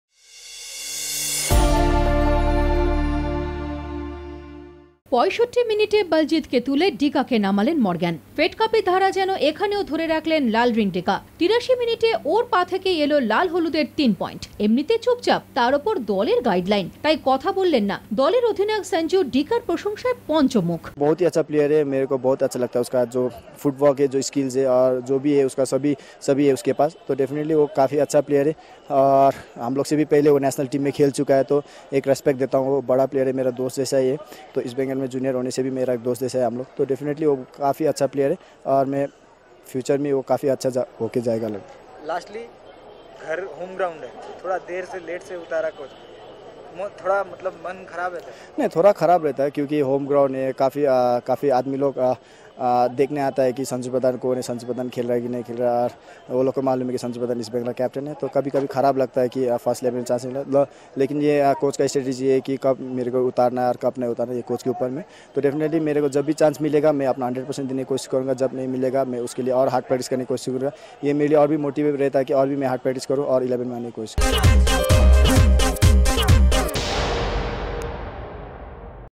I-LEAGUE 2012/13: EAST BENGAL VS UNITED SIKKIM... MATCH HIGHLIGHTS!
GANGTOK: I-League giant East Bengal struggled hard to break down a resilient newcomer United Sikkim FC before scoring a 1-0 victory with a freakish goal seven minutes before the final whistle on Thursday here at Paljor Stadium.

A fierce free kick from second half substitute Lalrindika Ralte in the 83rd minute ricocheted off a United Sikkim defender and flew past goalkeeper Yoon Tae for the only goal in a game of overwhelming dominance for East Bengal.

Hampered by niggling injuries to its key players, United Sikkim could not repeat its counterattacking performance like it did against Salgaocar in the first match and was bogged down in its own half against Trevor Morgan's boys. Clearly playing for a draw, the home side was contended in beefing up its defense, rarely got a glimpse of its opponent goal but also blocked the goal view for East Bengal. When asked to Sanju Pradhan, he said,"Dika is a great prospect, despite being younger, he represented India before we did. It tells the story, we respect him a lot."

The defensive tactics frustrated East Bengal players till the solitary goal, with a huge slice of luck, came along its way. Today's victory ensure that the Kolkata club maintains its 23 matches unbeaten run and the first full 3 points in I-League.